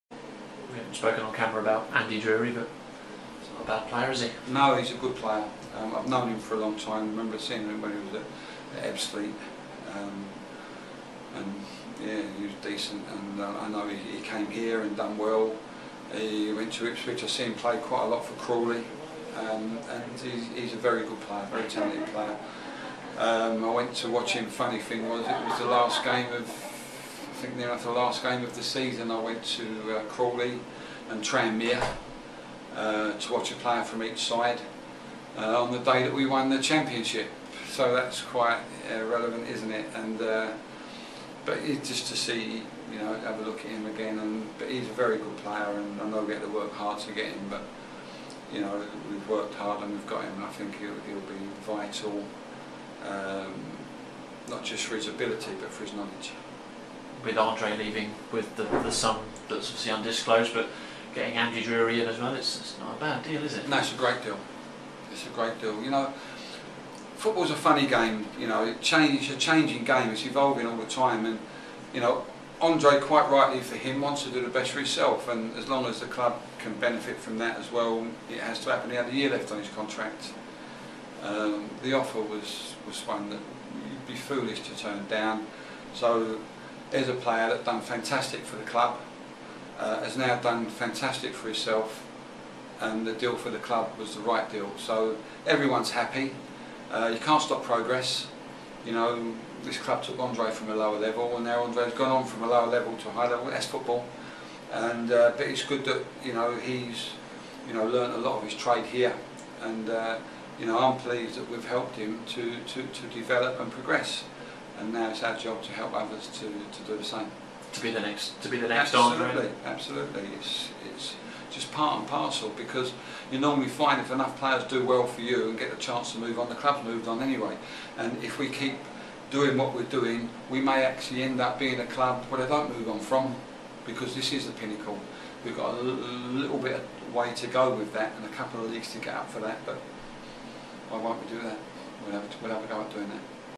John Still on Andy Drury and Andre Gray
The boss talks about the signing of Andy Drury and the sale of Andre Gray and what it means to Luton Town.